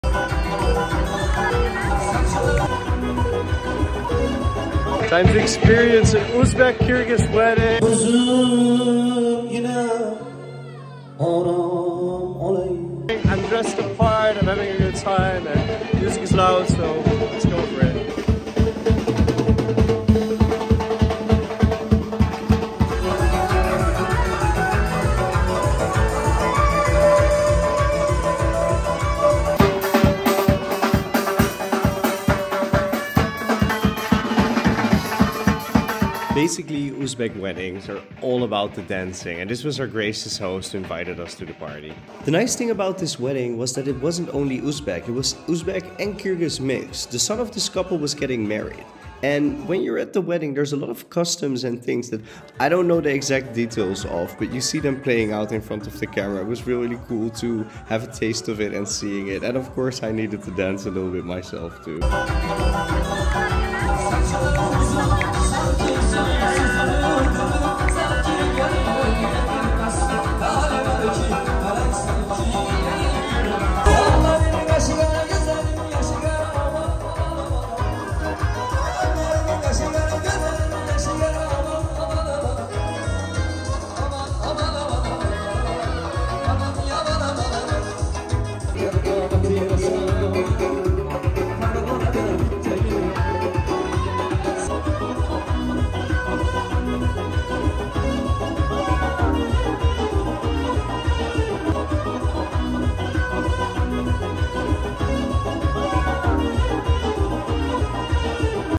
🇺🇿 Uzbek | Kyrgyz 🇰🇬 Wedding | DANCING WITH THE LOCALS
Uzbek music, traditions and Kyrgyz Music and traditions. Honestly, I had the best time ever at the dancefloor of this wedding. Tim is on YouTube too, find him at @a_freshstart